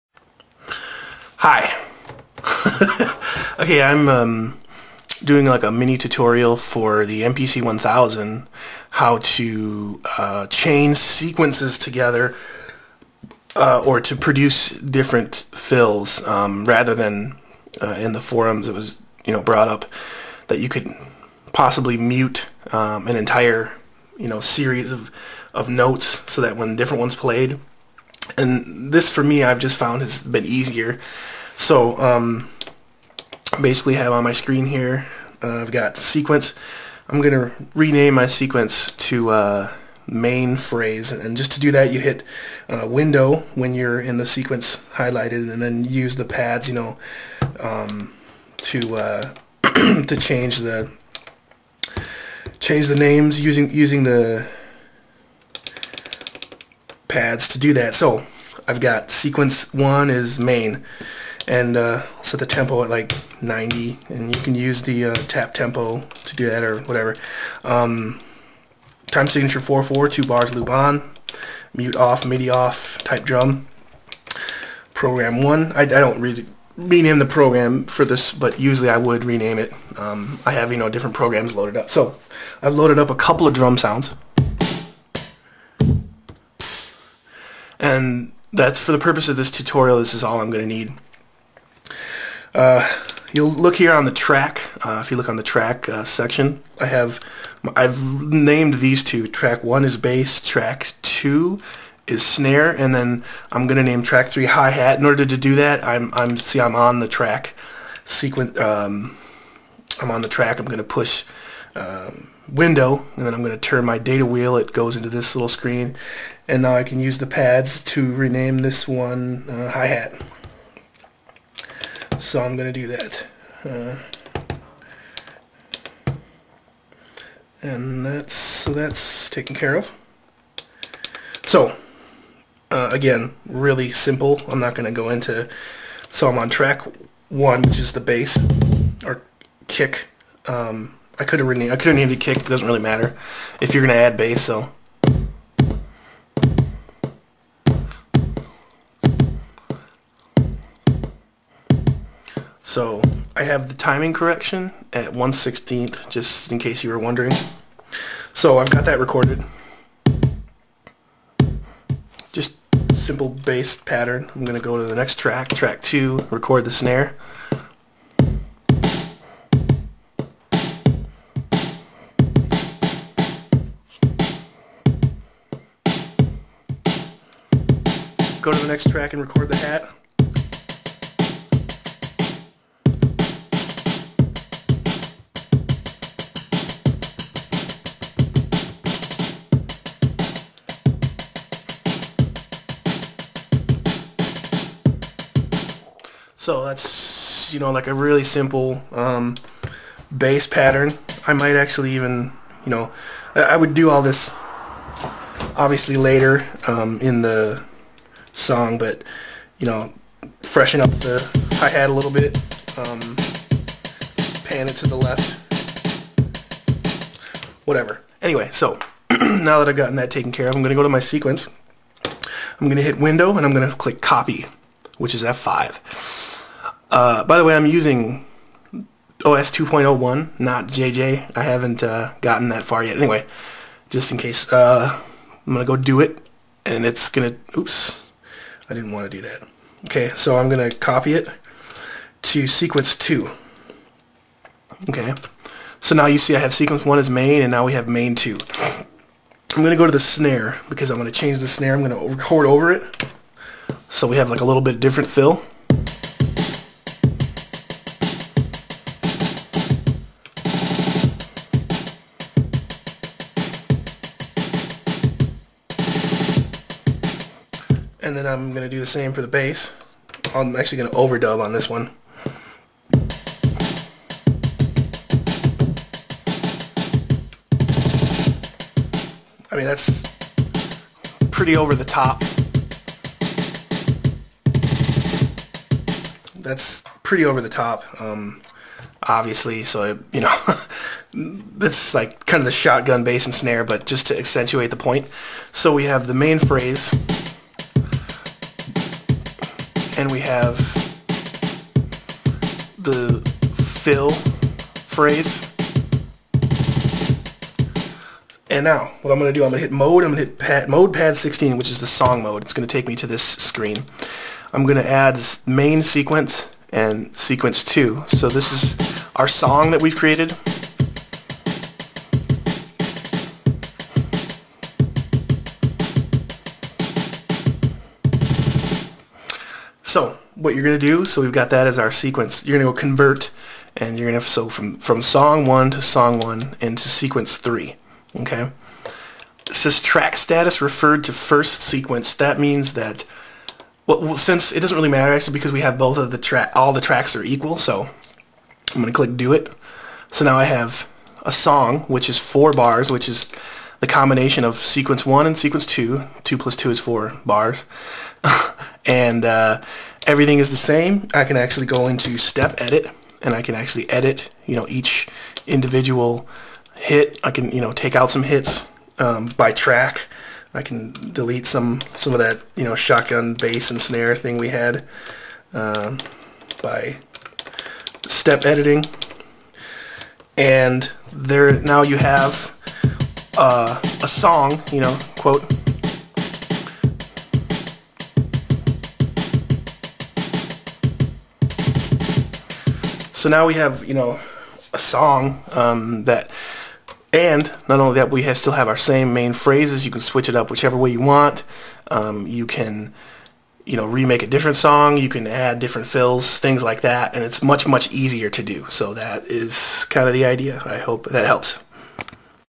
MPC Mini Tutorial
A basic tutorial on chaining sequences to make a song, and how to make different fills